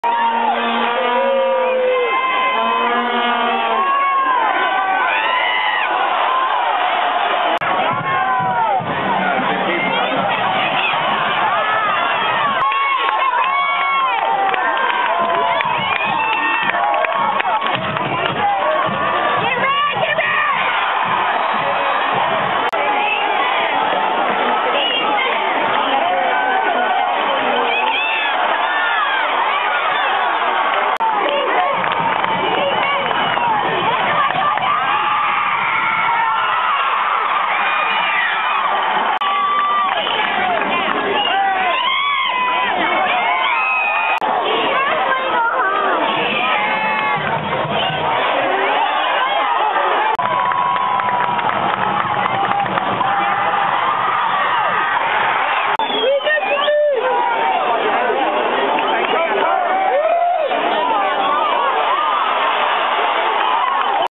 Cayleb Jones v. Belton.dv
2012 Austin High WR Cayleb Jones making big plays against Belton on September 3, 2010. Jones will probably be a top Texas target in 2012 and is one of the best players in his class, regardless of position.